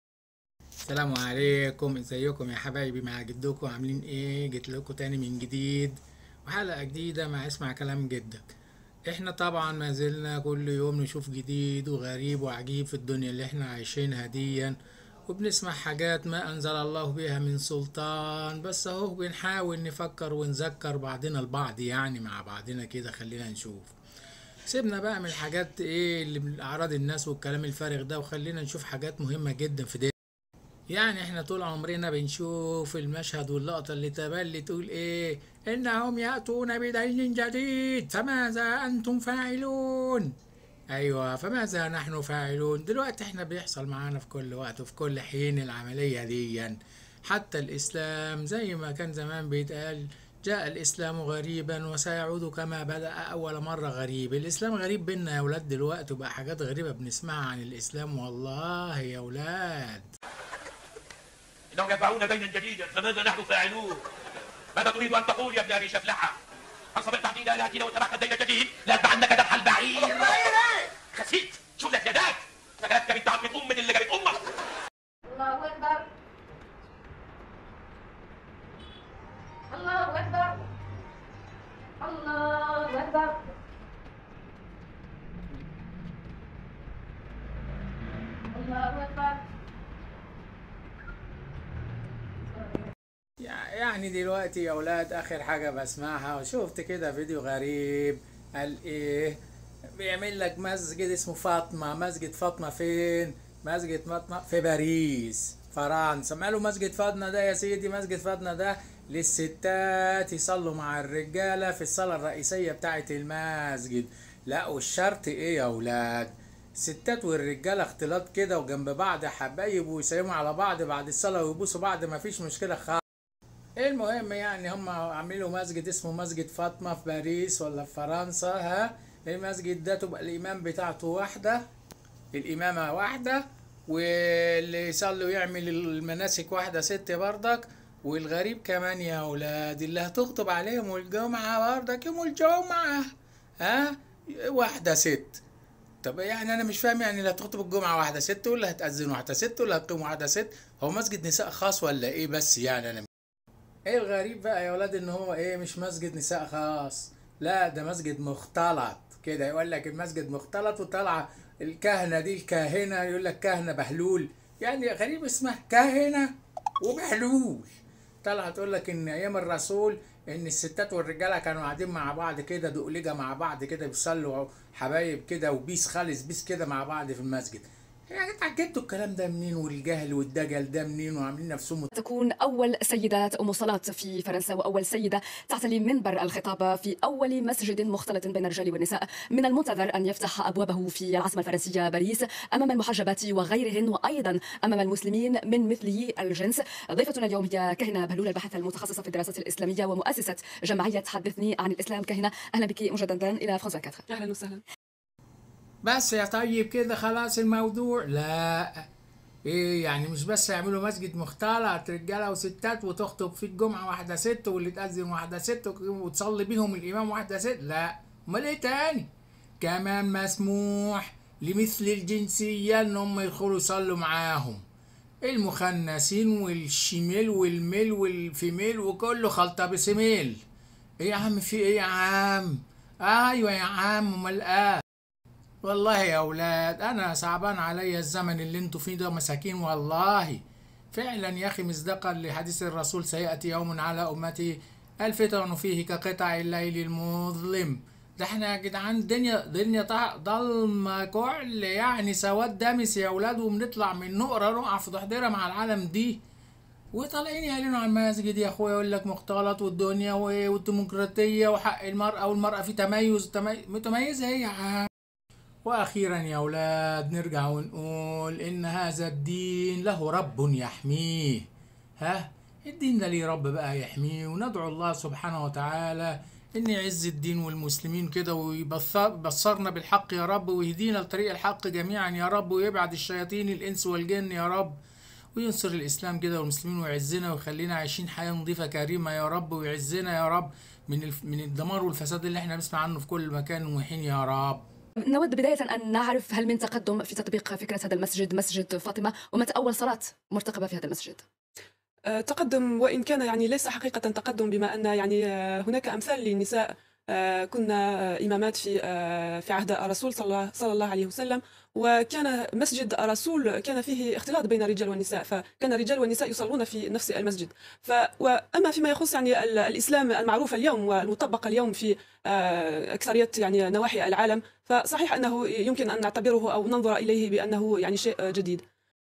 مسجد فاطمة المختلط في فرنسا.. مشروع أول مسجد تؤمه سيدة
يارب احمي دينك من الفسدة
نعم انهم يدعون الي دين جديد
الاختلاط والشواذ في بيت الله
ماذا بعد
قناة شاملة عامة تشمل المعلومة والفكرة والترفية والفكاهة والمرح  والطفل والاسرة
صفحة هيمة و شوشو علي انستجرام